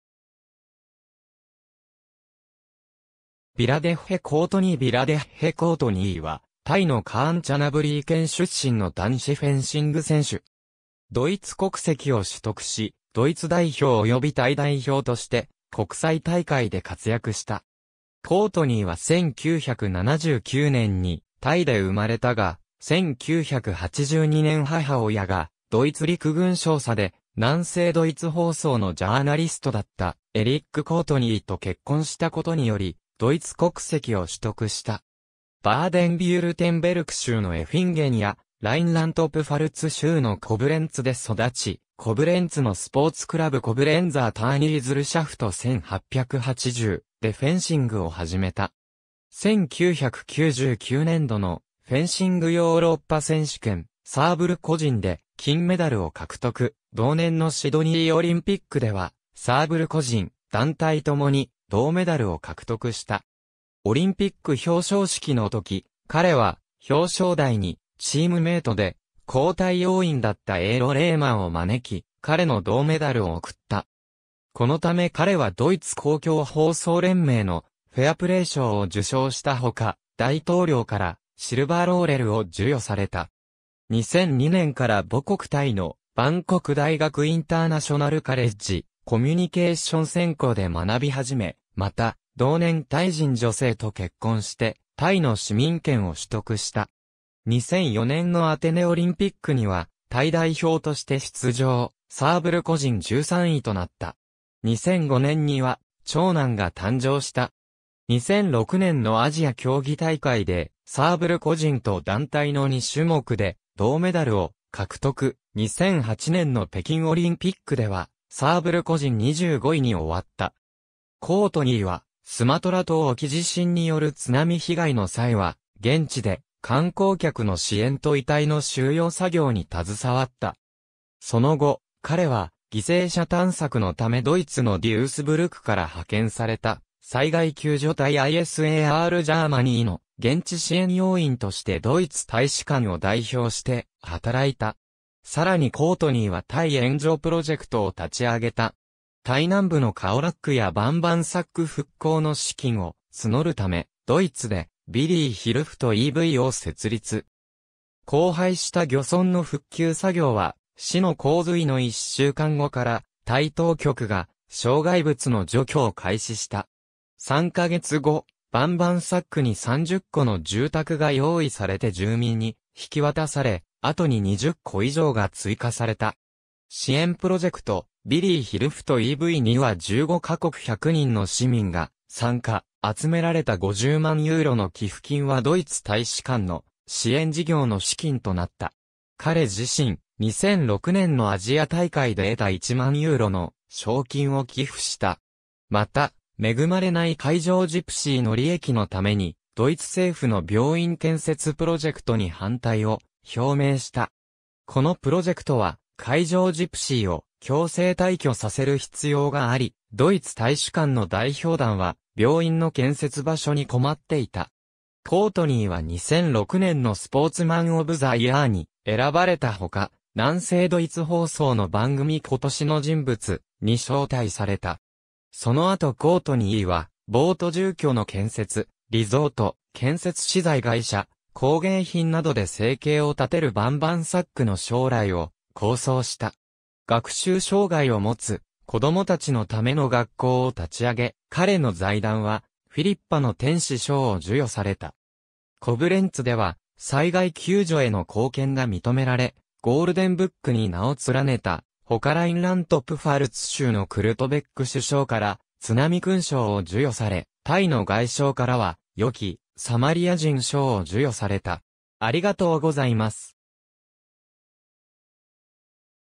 ヴィラデッヘ・コートニー
ヴィラデッヘ・コートニー ヴィラデッヘ・コートニー（Wiradech Kothny、1979年5月10日 – ）は、タイのカーンチャナブリー県出身の男子フェンシング選手。
ドイツ国籍を取得し、ドイツ代表及びタイ代表として国際大会で活躍した。
コートニーは1979年にタイで生まれたが、1982年母親がドイツ陸軍少佐で南西ドイツ放送（SWR）のジャーナリストだったエリック・コートニーと結婚したことによりドイツ国籍を取得した。
バーデン＝ヴュルテンベルク州のOeffingenやラインラント＝プファルツ州のコブレンツで育ち、コブレンツのスポーツクラブ"Coblenzer Turngesellschaft 1880"でフェンシングを始めた。
1999年度のフェンシングヨーロッパ選手権サーブル個人で金メダルを獲得、同年のシドニーオリンピックではサーブル個人、団体ともに銅メダルを獲得した。
オリンピック表彰式の時、彼は表彰台にチームメートで交代要員だったエーロ・レーマンを招き、彼の銅メダルを贈った。
このため彼はドイツ公共放送連盟（ARD）の"フェアプレー賞"を受賞したほか、大統領から"シルバーローレル"を授与された。
2002年から母国タイのバンコク大学インターナショナルカレッジ、コミュニケーション専攻で学び始め、また、同年タイ人女性と結婚してタイの市民権を取得した。
2004年のアテネオリンピックにはタイ代表として出場、サーブル個人13位となった。
2005年には長男が誕生した。
2006年のアジア競技大会でサーブル個人と団体の2種目で銅メダルを獲得、2008年の北京オリンピックではサーブル個人25位に終わった。
コートニーはスマトラ島沖地震 (2004年)による津波被害の際は現地で観光客の支援と遺体の収容作業に携わった。
その後、彼は犠牲者探索のためドイツのデュースブルクから派遣された災害救助隊I.S.A.R. Germanyの現地支援要員としてドイツ大使館を代表して働いた。
更にコートニーはタイ援助プロジェクトを立ち上げた。
タイ南部のカオラックやバンバンサック復興の資金を募るためドイツで"Willi hilft e. V."を設立。
荒廃した漁村の復旧作業は"死の洪水"の一週間後からタイ当局が障害物の除去を開始した。
3ヶ月後、バンバンサックに30戸の住宅が用意されて住民に引き渡され、後に20戸以上が追加された。
支援プロジェクト"Willi hilft e. V."には15カ国100人の市民が参加、集められた50万ユーロの寄付金はドイツ大使館の支援事業の資金となった。
彼自身、2006年のアジア大会で得た1万ユーロの賞金を寄付した。
また、恵まれない海上ジプシーの利益のためにドイツ政府の病院建設プロジェクトに反対を表明した。
このプロジェクトは海上ジプシーを強制退去させる必要があり、ドイツ大使館の代表団は病院の建設場所に困っていた。
コートニーは2006年のスポーツマン・オブ・ザ・イヤーに選ばれたほか、南西ドイツ放送の番組"今年の人物"に招待された。
その後コートニーはボート住居の建設、リゾート、建設資材会社、工芸品などで生計を立てるバンバンサックの将来を構想した。
学習障害を持つ子供たちのための学校を立ち上げ、彼の財団は"フィリッパの天使"賞を授与された。
コブレンツでは災害救助への貢献が認められ、"ゴールデン・ブック"に名を連ねたほかラインラント＝プファルツ州のクルト・ベック首相から"津波勲章"を授与され、タイの外相からは"よきサマリア人"賞を授与された。